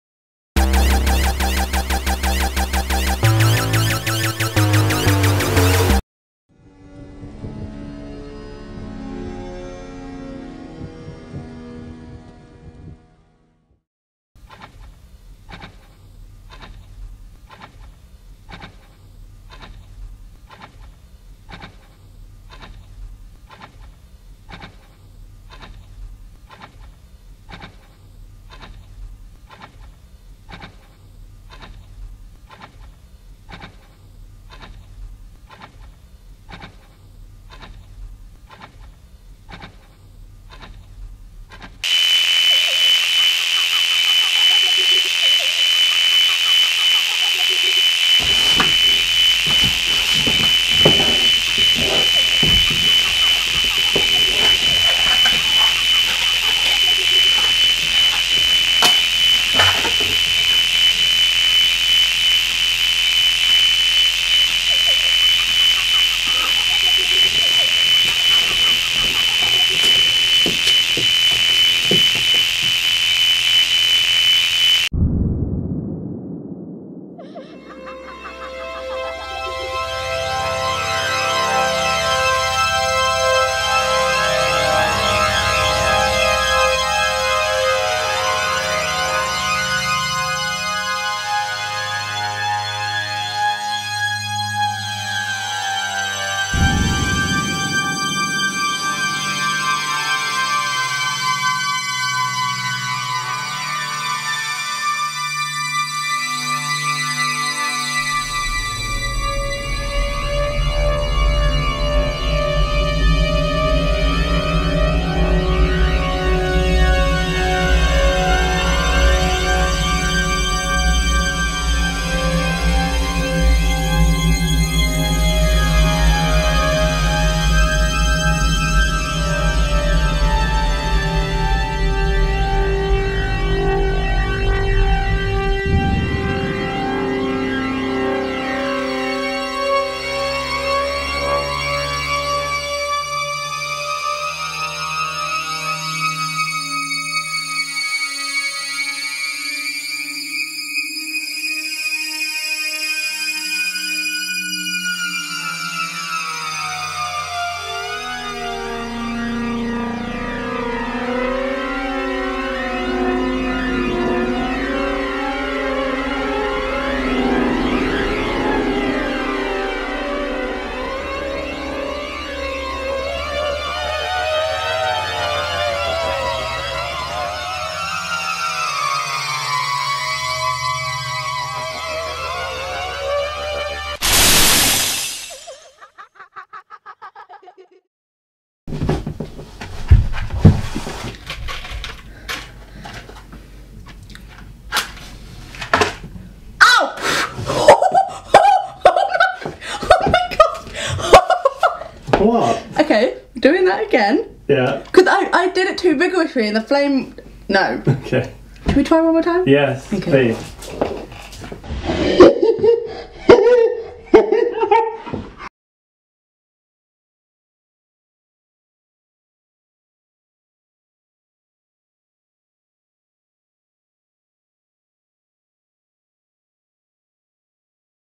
Samo Uninhibited: Shorts - 04 Mirror
A lonely girl sits at home alone by a radio, waiting for something...